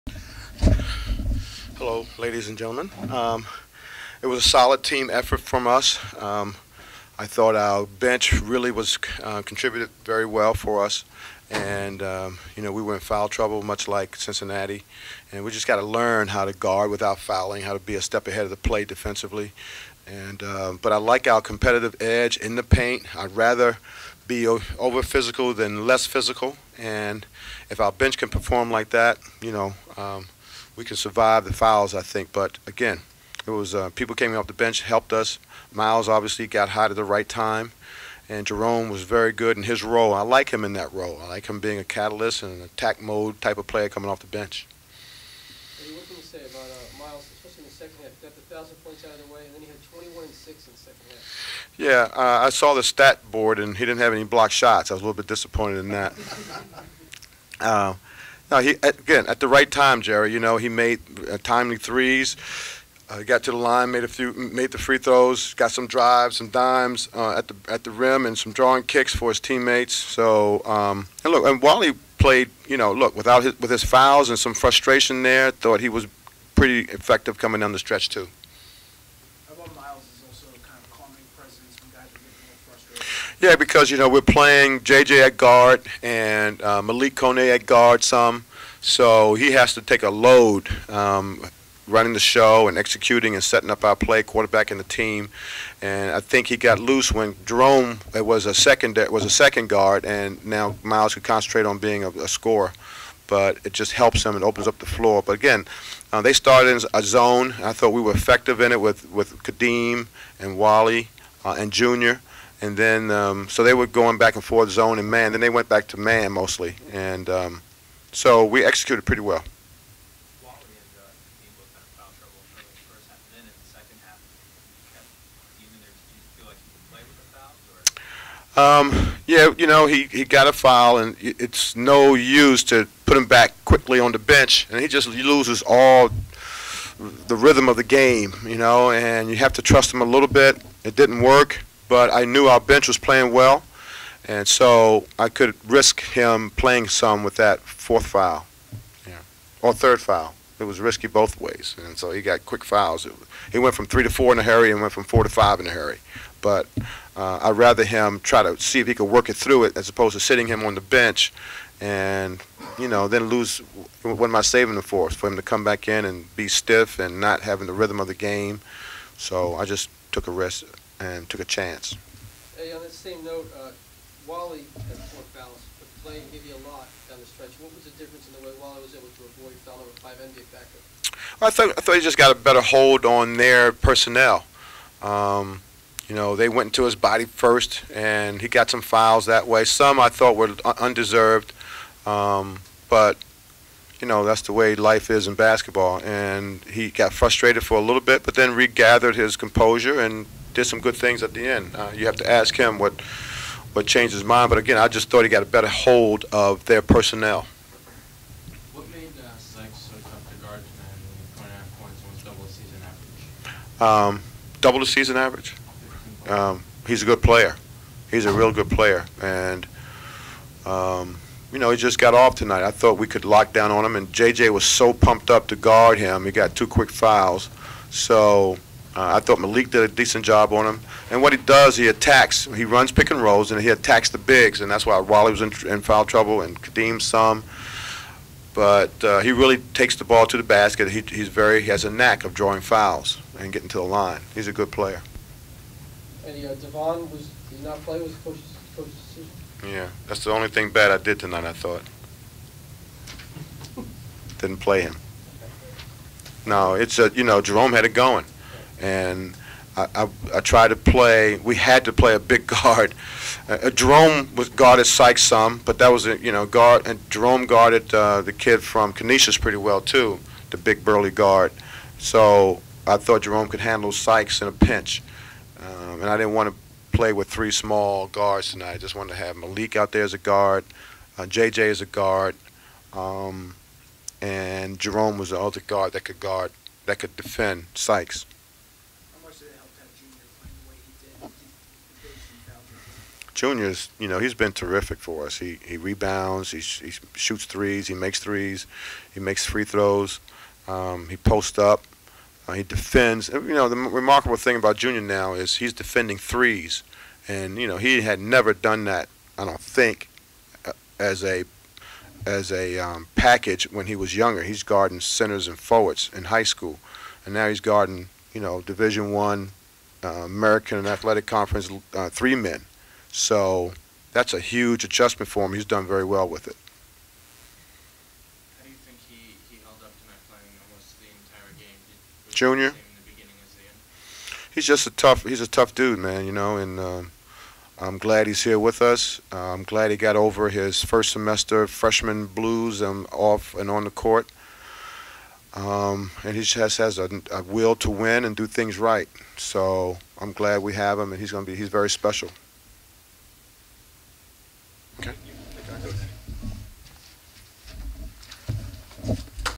RVision: @EddieJordanRU UCF Postgame
Head coach Eddie Jordan spoke with members of the media following Rutgers' 85-75 victory over UCF.